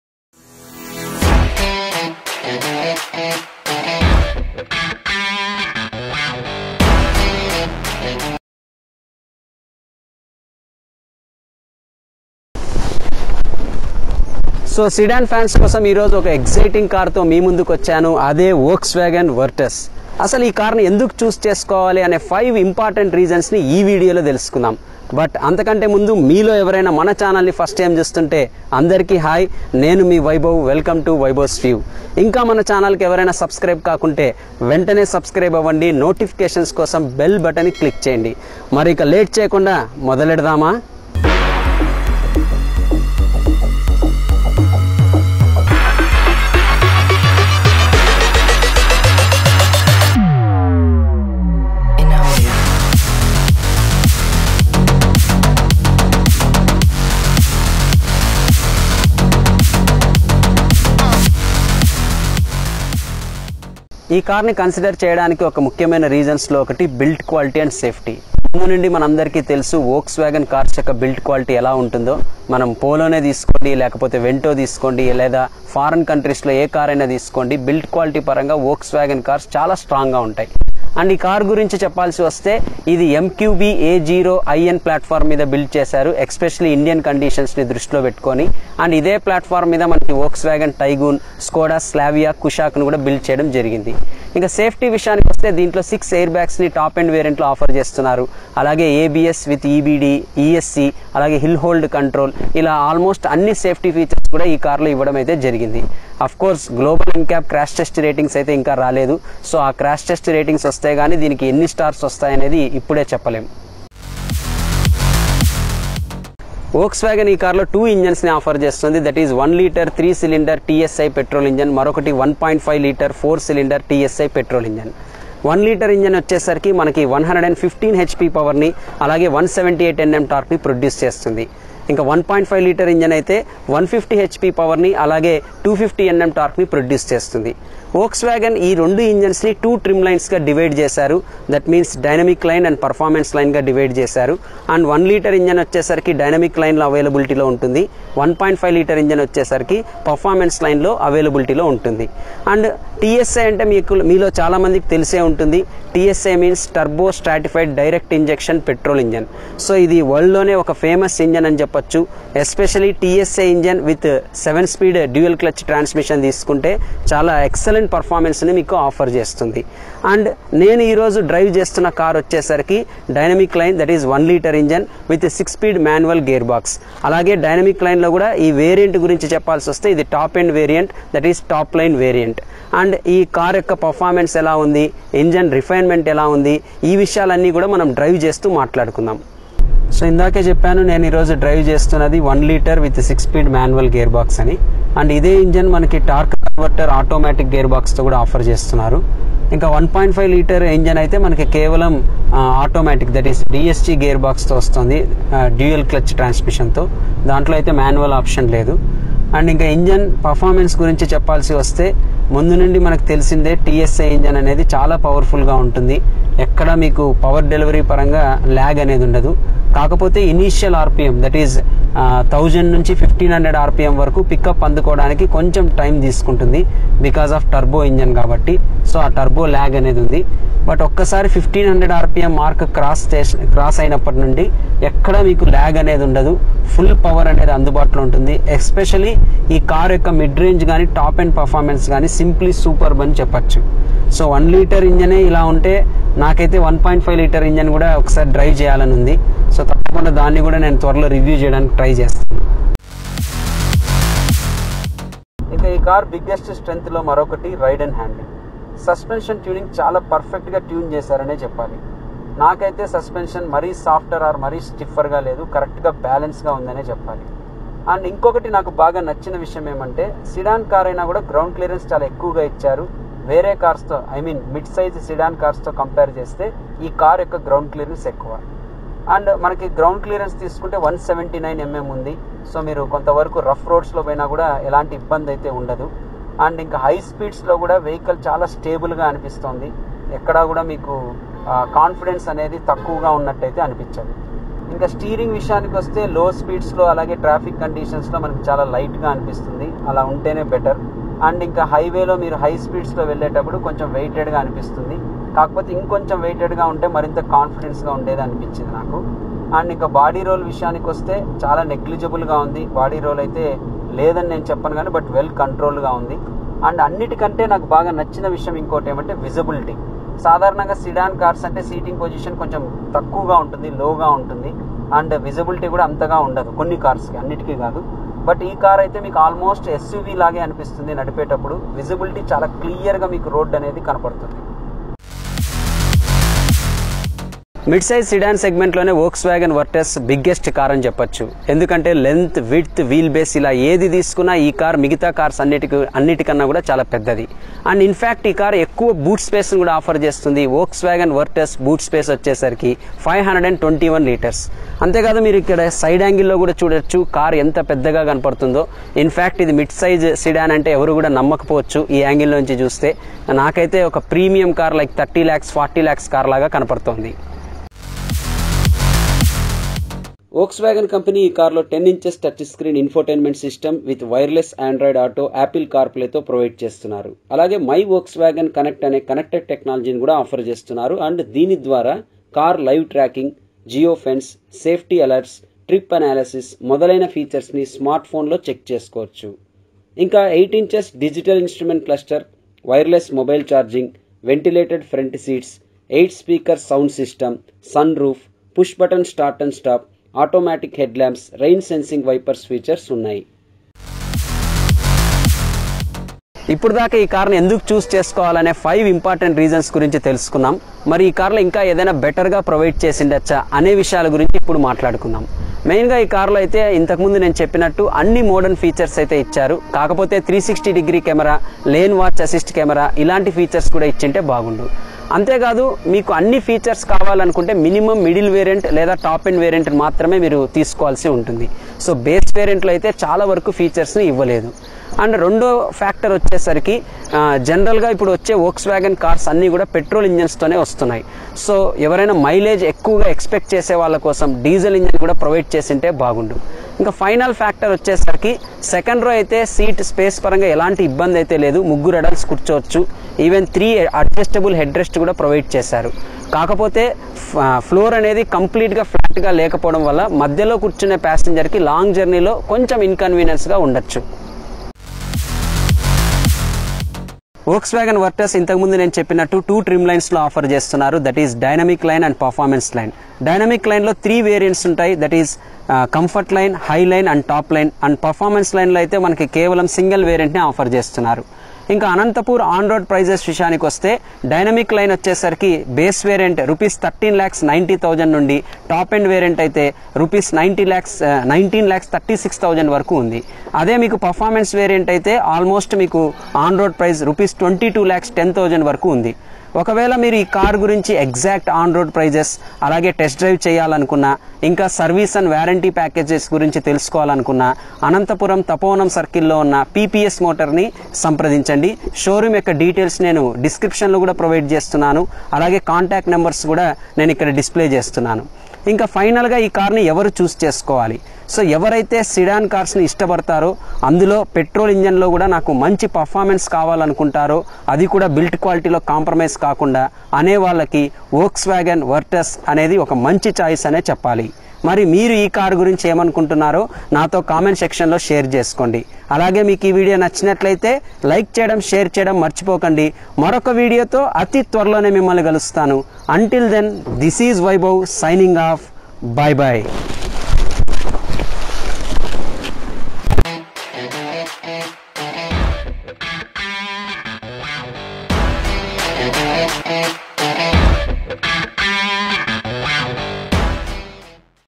Volkswagen Virtus Detailed Review in Telugu I New King in Mid Size Segment! I Vaibhavs View
Volkswagen Virtus - Virtus is a new mid size sedan from Volkswagen. It is built on MQB A0 IN platform which is exclusively designed for India. Volkswagen Virtus comes with two turbo charged petrol engines and two gearboxes. Virtus is also equipped with all modern features.

In this video we are going to tell you what are the five important reasons to consider Volkswagen Virtus and also tell you the on road prices of this car. So watch the video completely.

0:12 - Introduction
1:01 - Safety & Built Quality
1:53 - Engine & Performance
4:12 - Ride & Handling
5:34 - Boot Space & Dimensions
6:07 - Features
6:43 - What Could be Better
07:44 - Verdict & On Road Prices

Mileage as Per ARAI
1.0 TSI MT 19.40 kmpl
1.0 TSI AT 18.12 kmpl
1.5 TSI DSG 18.67 kmpl

Volkswagen Anantapur Showroom Details:
No: 3-454,
Near 7H Car Decors,
Tapovanam,
Anantapur, Andhra Pradesh-515001